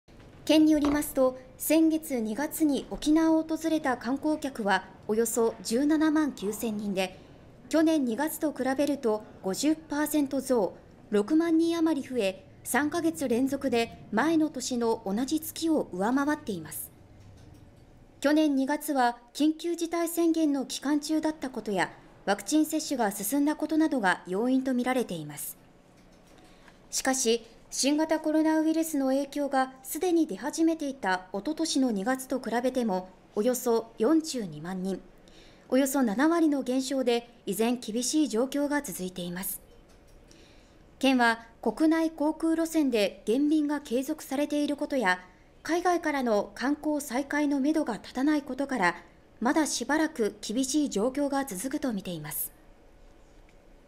RBC NEWS「2月の入域観光客数 3か月連続の増加」2022/03/26
先月２月に
沖縄を訪れた観光客は
およそ１７万９０００人で、
去年２月と比べ、
およそ５割増えています